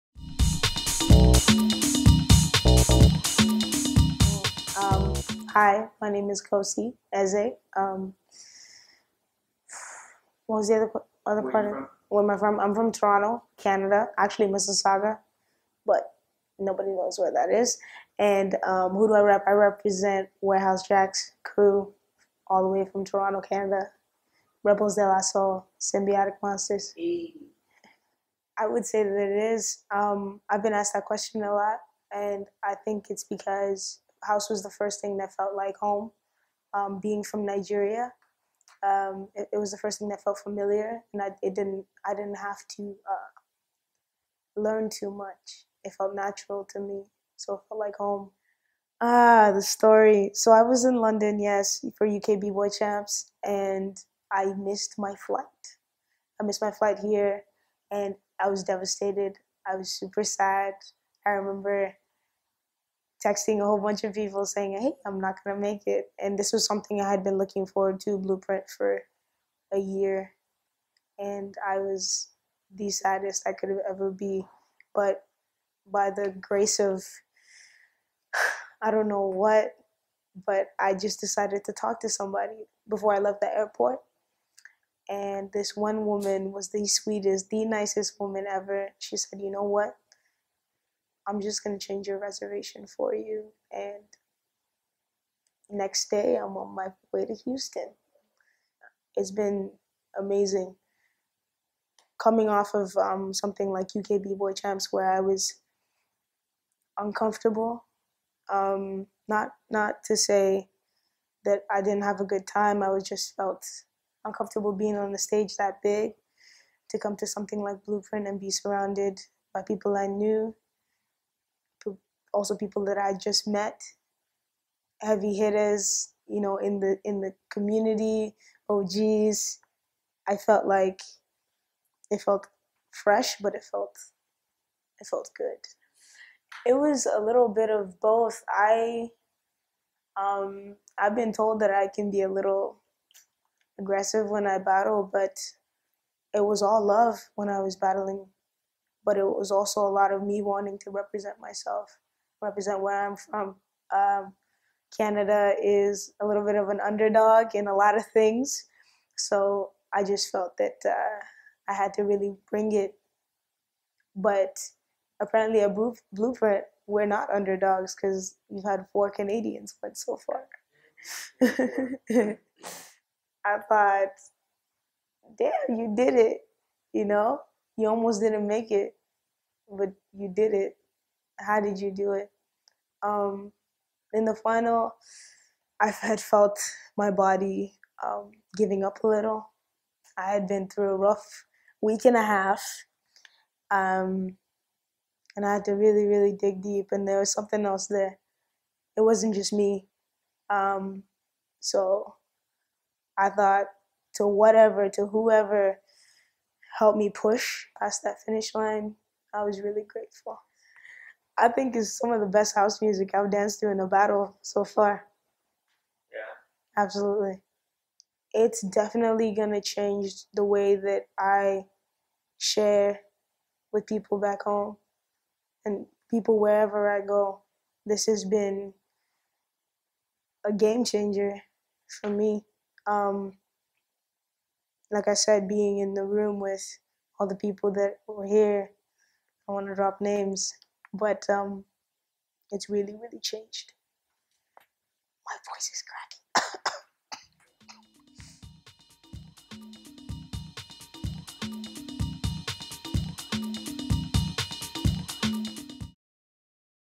The Blueprint 2017 - Kosi aka KC Interview
THE BLUEPRINT 2017 - HOUSE DANCE COMPETITION
[HOUSTON, TX - UNITED STATES]
Caleaf & Coflo Interview

The Blueprint is an annual House Dance experience that takes place in Houston Texas.

Organized by: Damian Lopez & Noey Lopez